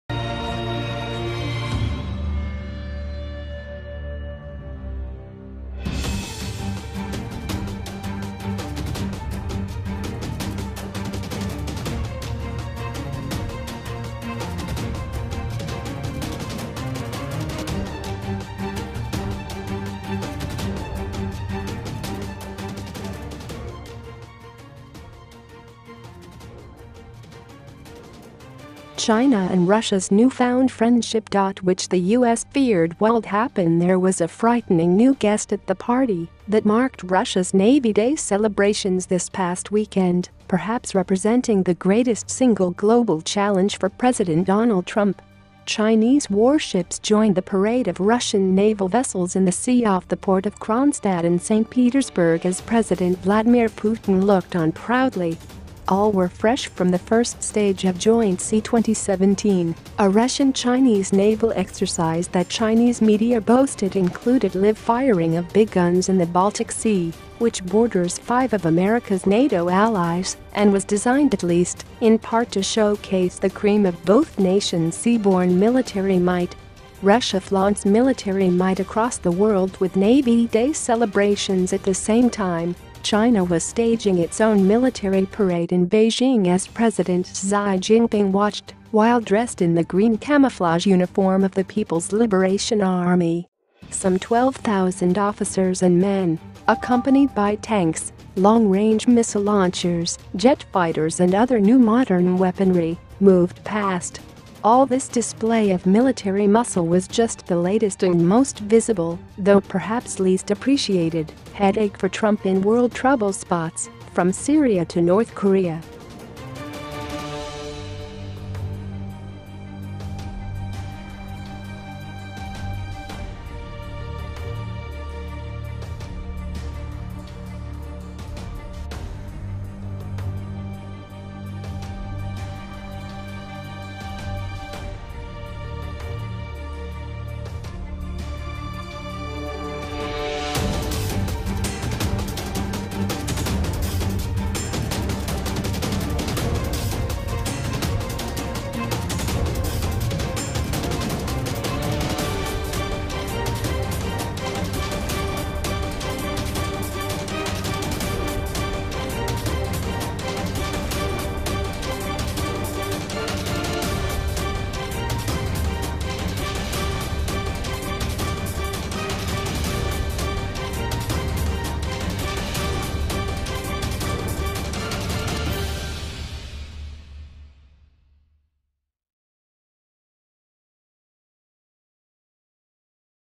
China and Russia's newfound friendship which the u s feared wuld happen
There was a frightening new guest at the party that marked Russia's Navy Day celebrations this past weekend -- perhaps representing the greatest single global challenge for President Donald Trump.